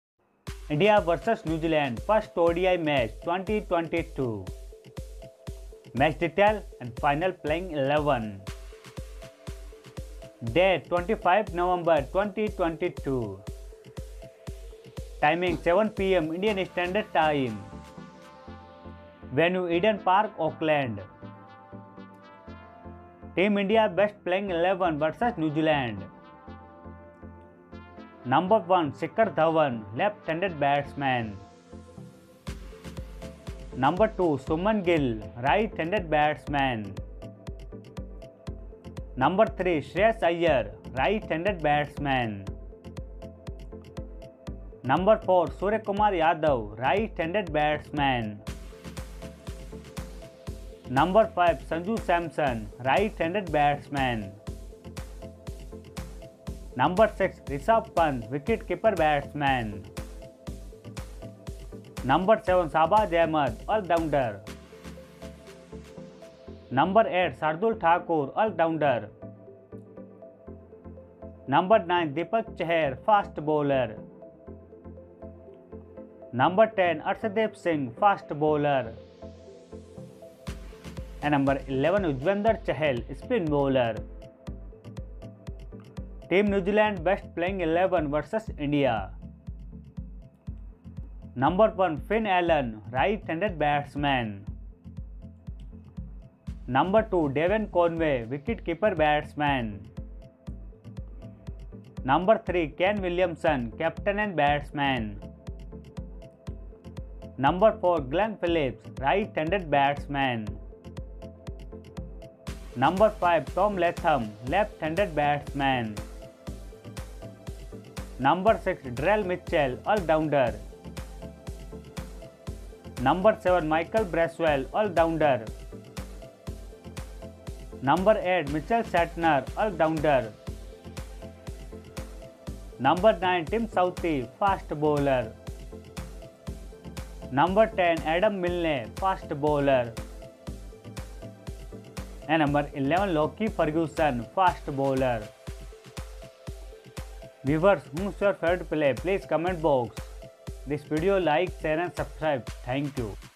India vs New Zealand 1st ODI Playing 11 | Ind Playing 11 vs Nz Playing 11
Match Details

India vs New Zealand 1st ODI Match
Venue- Bled Park, Auckland
Date & Time - November 25 at 07:00 AM IST

India Playing 11 vs New Zealand

Shikhar Dhawan, Shubman Gill, Sheryas Iyer, Suryakumar Yadav, Sanju Samson, Rishabh Pant, Shahabaz Ahmad, Shardul Thakur, Deepak Chahar, Arsdeep Singh, Yuzi Chahal

New Zealand Playing 11 vs India

Finn Allen, Devon Conway, Kane Williamson, Glenn Philips, Tom Latham, Daryl Mitchell, M. Braswell, Michel Sentaner, Tim Southee, Lockie ferguson, Adam Milne